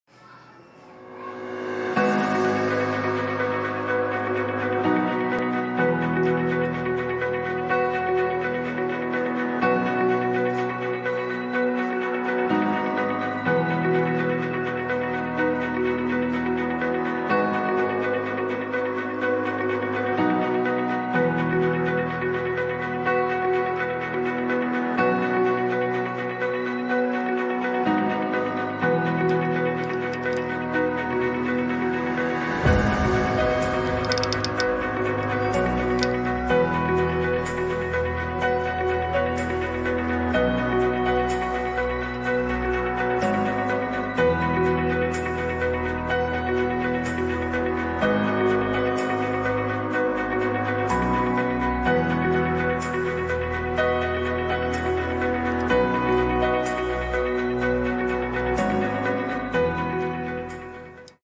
2006D Lincoln pennies Mint Error Close AM-D.Die-D.ear Found pocket change To look For !!
2006D# Lincoln pennies Mint Error# Close AM-D.Die-D.ear Found pocket change# To look For#